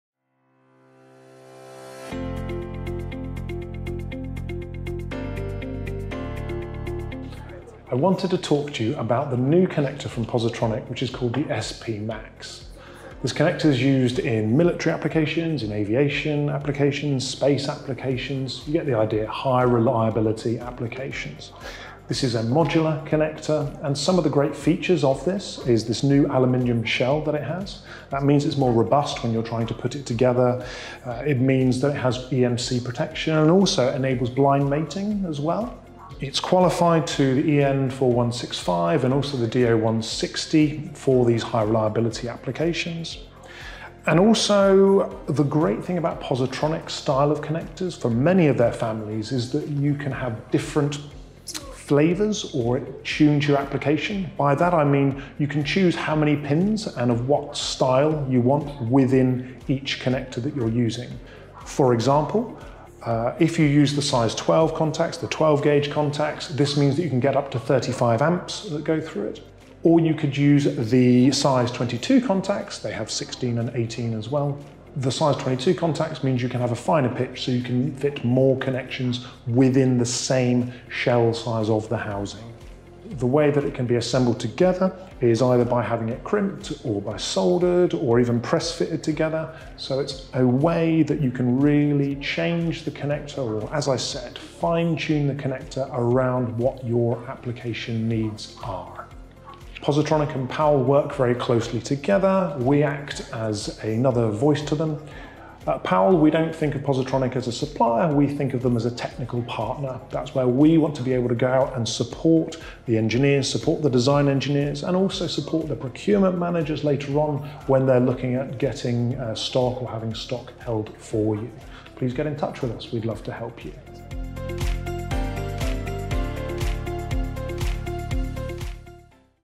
The SP Max from Positronic
A family of rugged power and signal connectors made to perform in the most extreme environments. for more information.